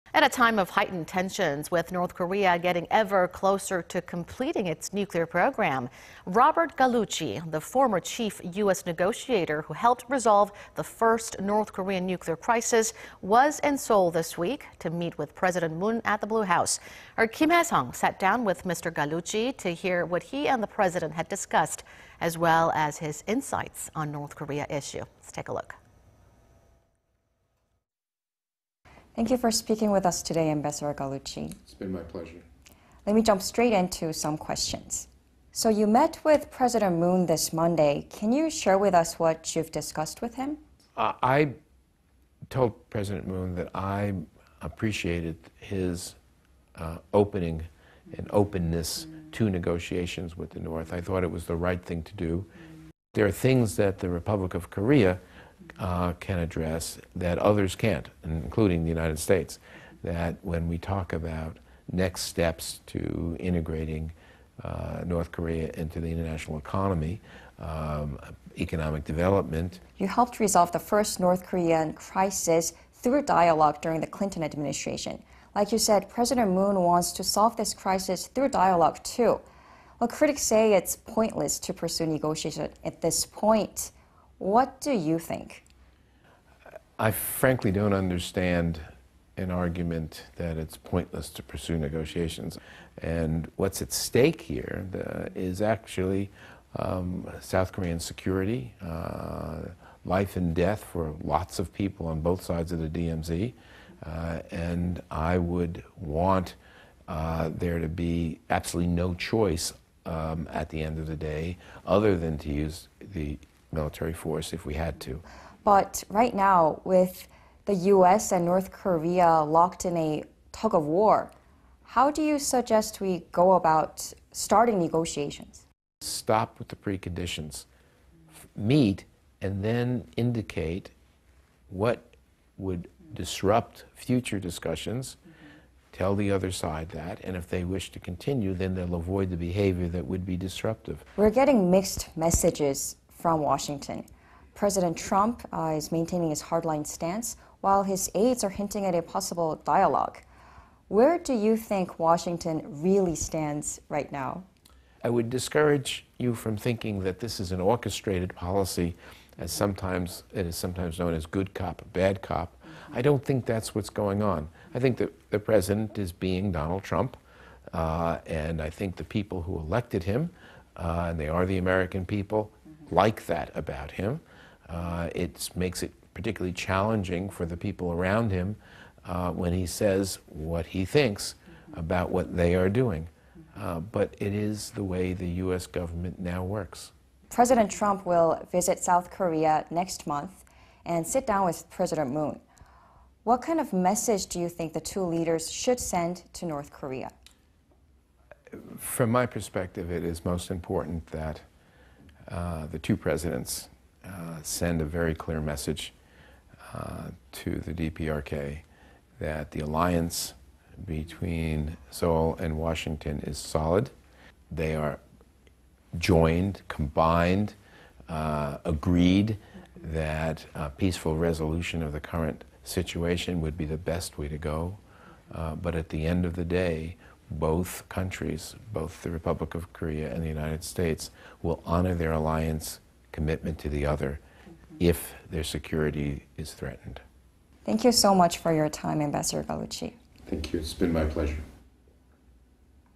Interview with Former Ambassador Robert Gallucci
갈루치 전 북핵 특사 대담
  At a time of heightened tensions,... with North Korea getting ever closer to completing its nuclear program,... Robert Gallucci, the former chief U.S. negotiator who helped resolve the first North Korean nuclear crisis, was in Seoul this week to meet with President Moon at the Blue House.
Our Kim Hyesung sat down with Mr. Gallucci to hear what he and the president had dicussed as well as his insights on North Korea issue.

  Thank you for speaking with us today, Ambassador Gallucci.

  It's my pleasure.

  Let me jump straight into some questions.

 1. So you met with President Moon this Monday. Can you share with us what you've discussed with him?

   I told President Moon that I appreciated his opening and openness to negotiations with the North.   I thought it was the right thing to do. There are things that the Republic of Korea can address that others can't, including the U.S. When we talk about next steps to integrating North Korea into the international economy, economic development...

2. You helped resolve the first North Korean crisis through dialogue during the Clinton Administration. Like you said, President Moon wants to solve this crisis through dialogue too. But critics say it's pointless to pursue negotiations at this point. What do you think?

  I frankly don't understand an argument that it's pointless to pursue negotiations. What's at stake here is actually South Korean security, life and death for lots of people on both sides at the DMZ.
And I would want there to be absolutely no choice, at the end of the day, other than to use military force if we had to.

3. But right now, with the U.S. and North Korea locked in a tug of war. How do you suggest we go about starting negotiations?

  Stop with the preconditions, meet and then indicate what would disrupt future discussions, tell the other side that, and if they wish to continue, then they will avoid the behavior that would be destructive.

4. We are getting mixed messages from Washington. President Trump is maintaining a hard line stance while his aides are hinting at a possible dialogue. Where do you think Washington really stands right now?

   I would discourage you from thinking that this is an orchestrated policy. It is sometimes known as good cop, bad cop, I don't think that's what's going on. I think the president is being Donald Trump,  and I think the people who elected him, the American people, like that about him. It makes it particularly challenging for the people around him when he says what he thinks about what they are doing. But it is the way the U.S. government now works.

5. President Trump will visit Korea next month and sit down with President Moon. What kind of message do you think the two leaders should send to North Korea?

   From my perspective it is most important that the two presidents send a very clear message  to the DPRK that the alliance between Seoul and Washington is solid.

They are joined, combined, agreed that a peaceful resolution of the current situation would be the best way to go. But at the end of the day,  both countries, both the R.O.K and the U.S. will honor their alliance commitment to the other if their security is threatened.

  Thank you so much for your time, Ambassador Gallucci.
  Thanks you, it's been my pleasure.